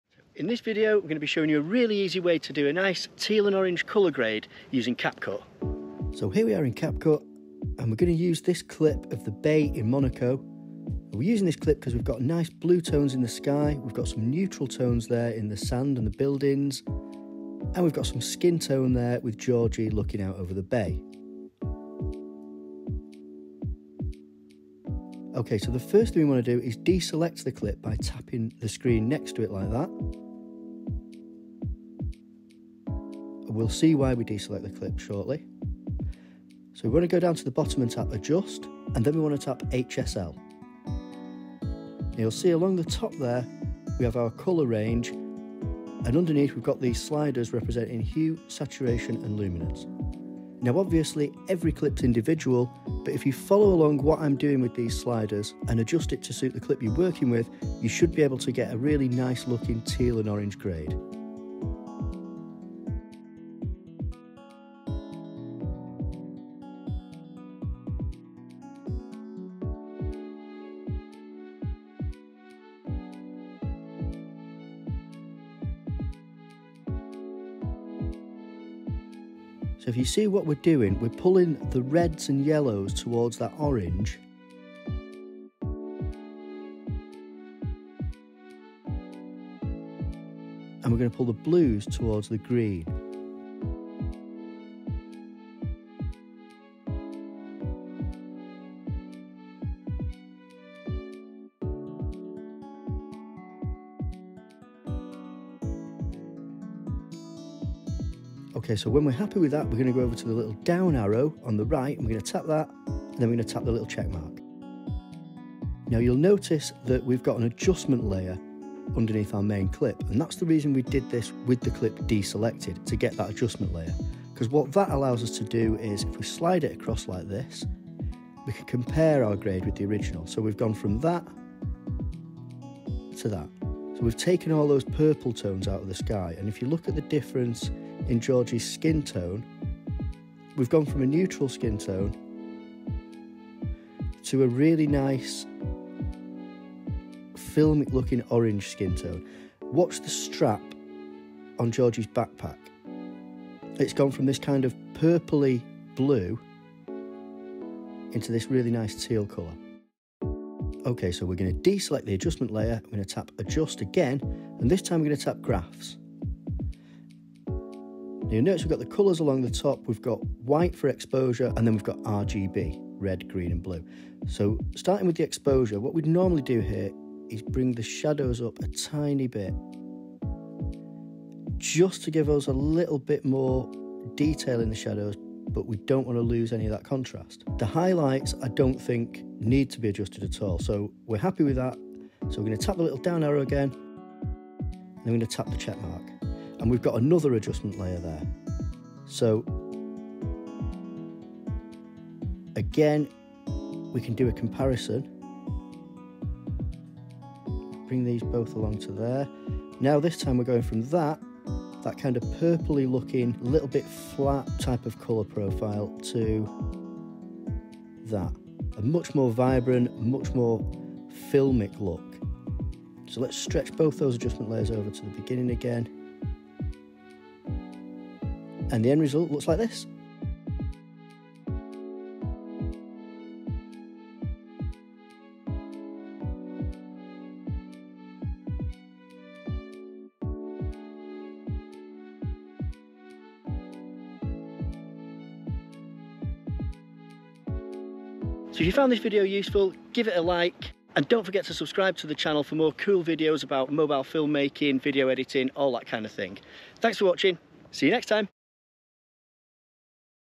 How to get a Teal & Orange Colour Grade in CapCut
Capcut is a free app but it has really good colour science and in this video we look at an easy method of achieving a nice Teal and Orange colour grade.

Chapters
00:00 Intro
00:36 Adjust
02:10 Compare
02:43 Graphs
03:54 Final Result